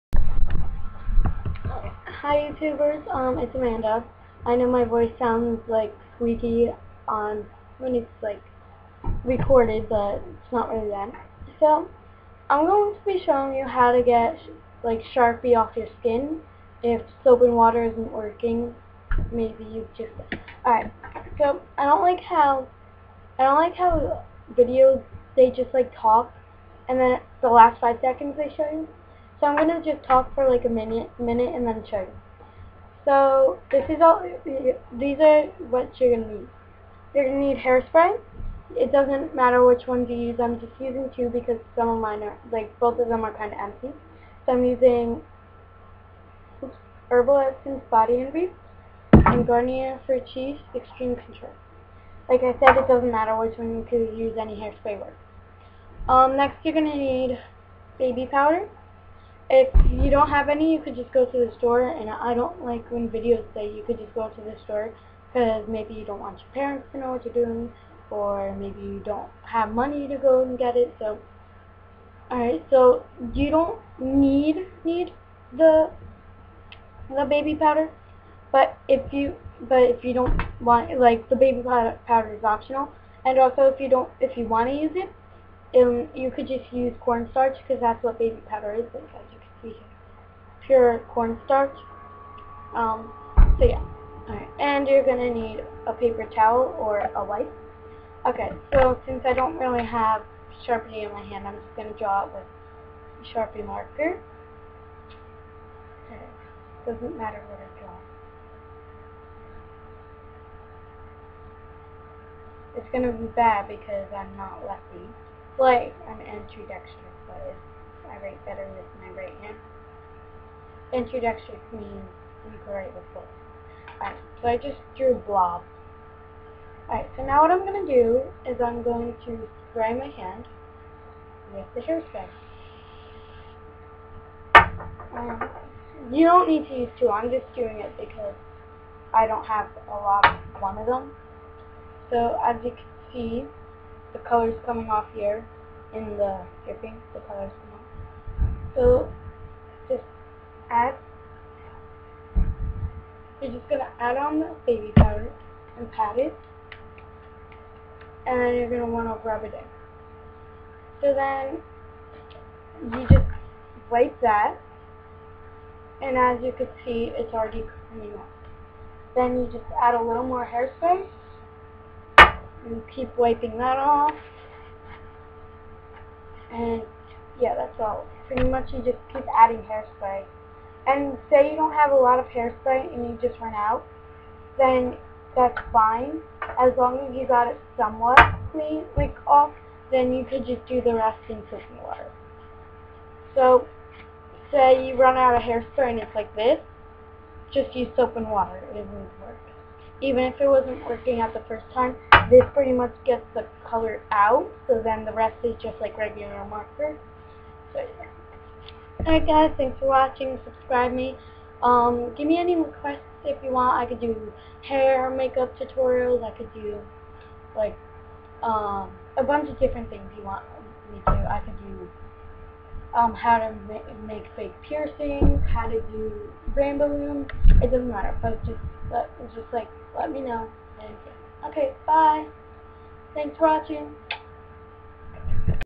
how to get sharpi off
This is how to get sharpie off your skin encase you arent aloud to have sharpie on yourself and you cant get it off with soap and water.